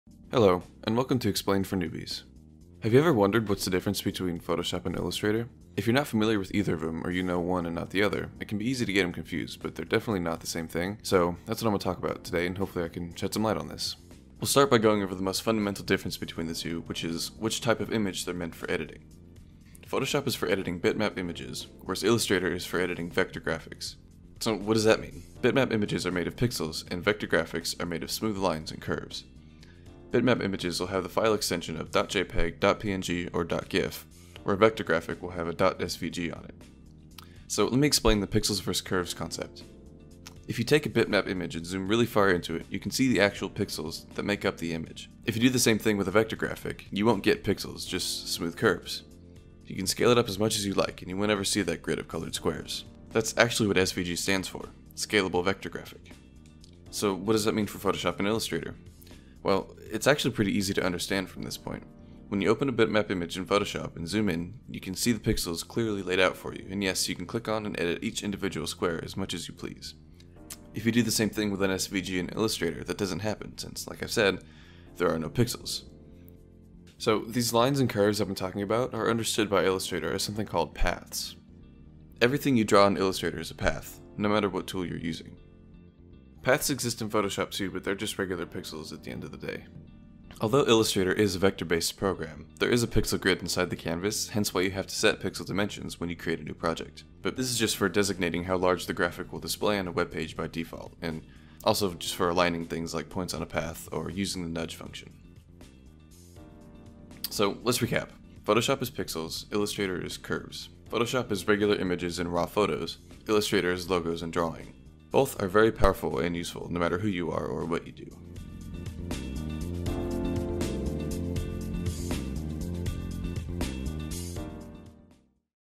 The difference between Photoshop and Illustrator
I made this video waaay back in college when I had too much free time on my hands. The whole "Explained for Newbies" thing came from about 5 seconds of thought, so I've scrapped that. I'm going to leave this video up though, purely for nostalgia despite the fact that "explained for newbies" makes me cringe